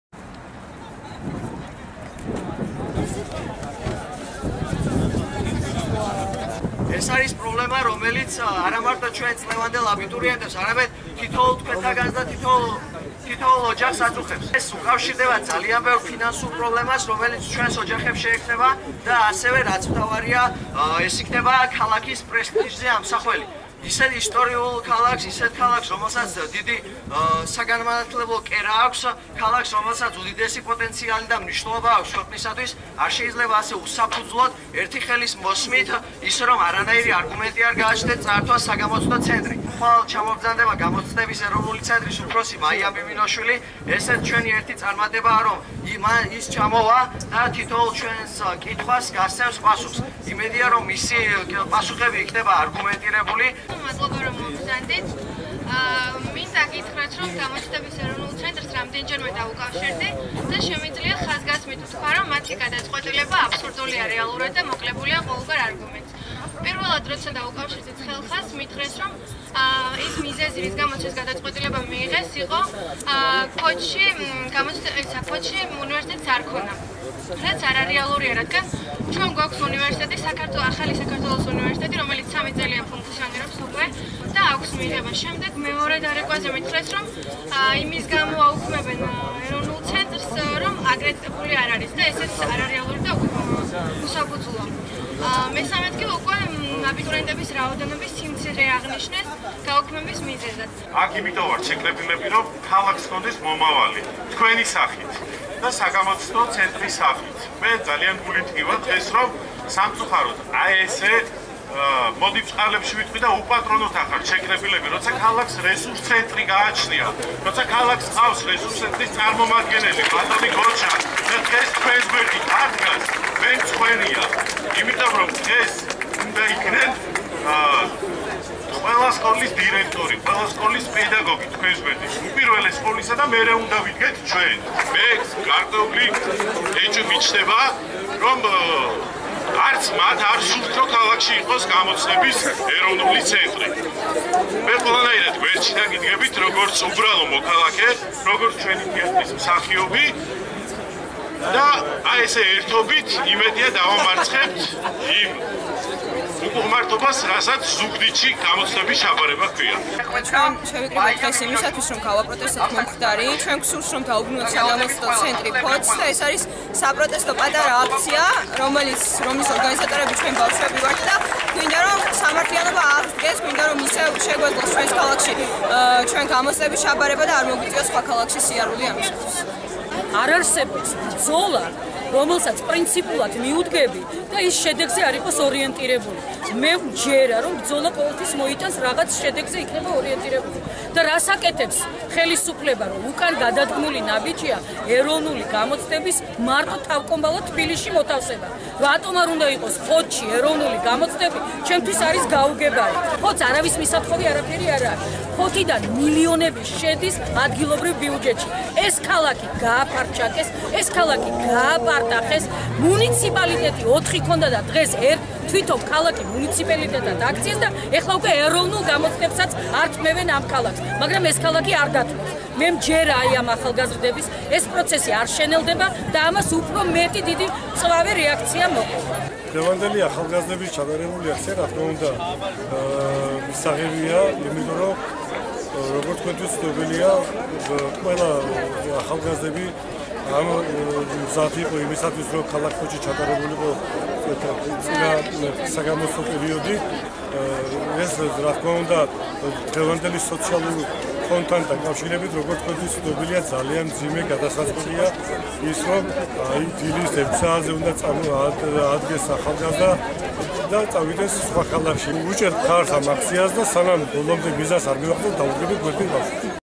აბიტურიენტთა აქცია ფოთში
ფოთელი აბიტურიენტები საგამოცდო ცენტრის ზუგდიდში გადატანას ეწინააღმდეგებიან და ფოთში დატოვებას ითხოვენ